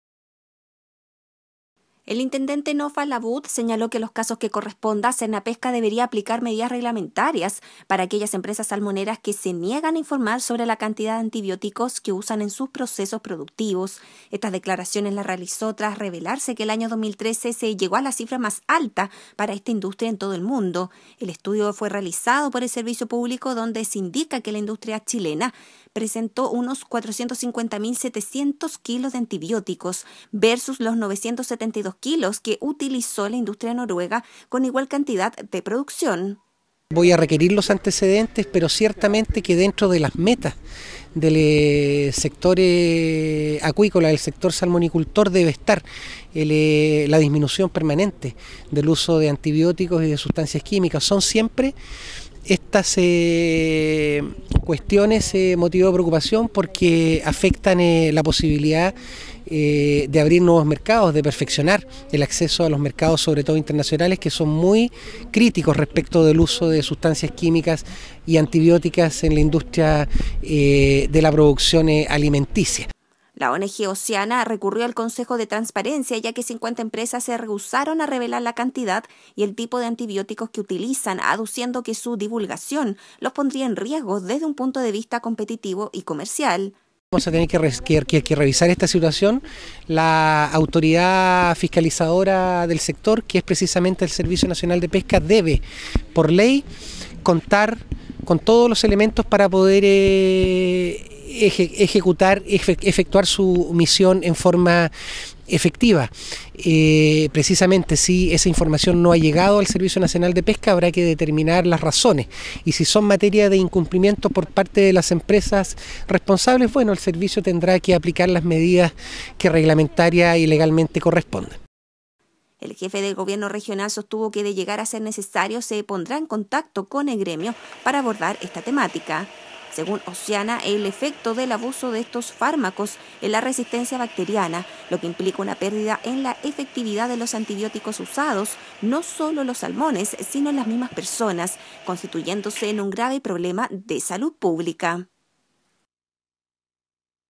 Critican negativa de salmoneras a informar uso de antiobióticos
El Intendente Nofal Abud señaló que en los casos que corresponda, Sernapesca debería aplicar medidas reglamentarias para aquellas empresas salmoneras que se niegan a informar sobre la cantidad de antibióticos que usan en sus procesos productivos. Esto tras revelarse que el año 2013 se llegó a la cifra más alta para esta industria en todo el mundo.